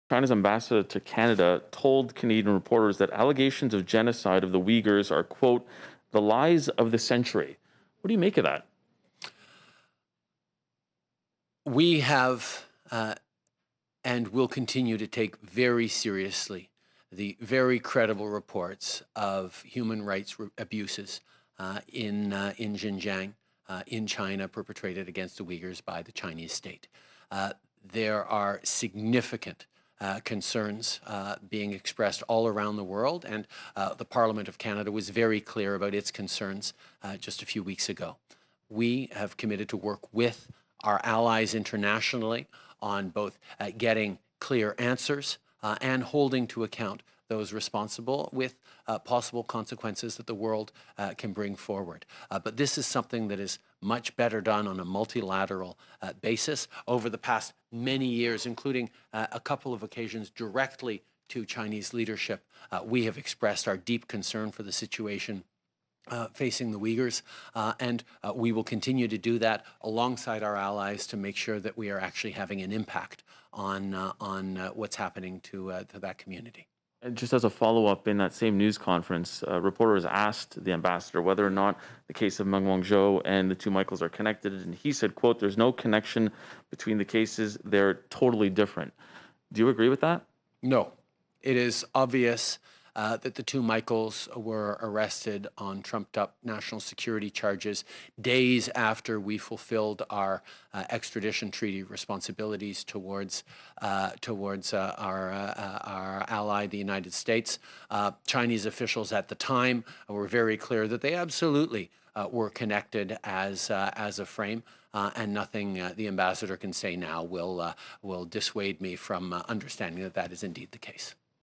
Trudeau snaps back after Chinese ambassador's comments
Prime Minister Justin Trudeau responded after the Chinese ambassador to Canada claimed Michael Spavor and Michael Kovrig were arrested for "undermining China's national security."

Here's what he had to say.